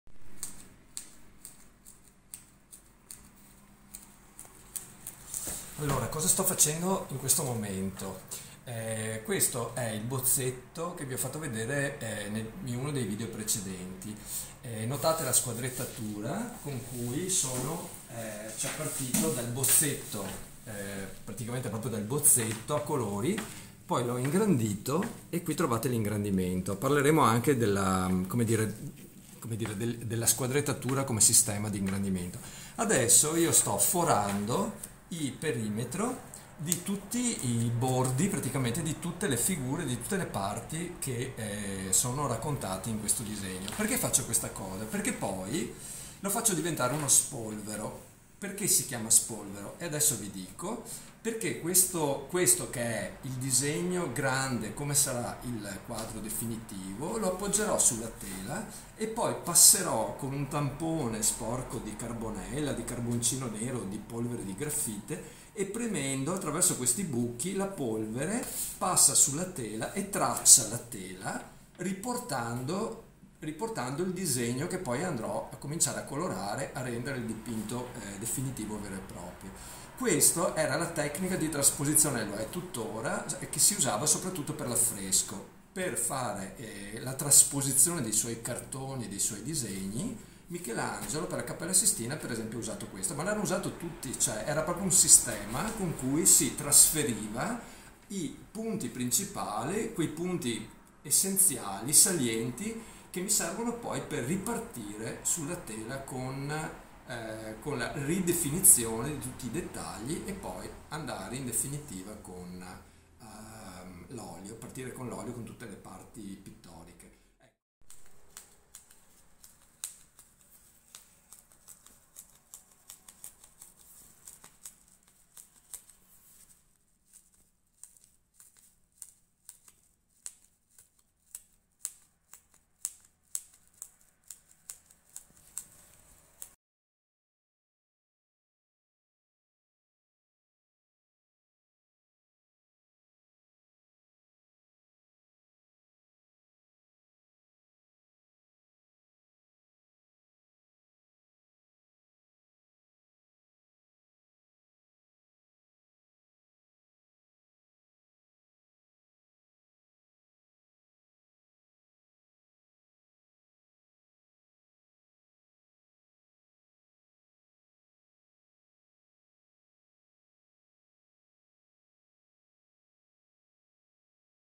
LO SPOLVERO _ UN PERCORSO SULL'ARTE E NELL'ARTE DA DENTRO IL MIO ATELIER
Lo spolvero è una tecnica di trasposizione di un disegno grande quanto l'opera definitiva su una tela o tavola. In questo video potete vedere come viene effettuato nella pratica.
Scusate l'audio un po' basso.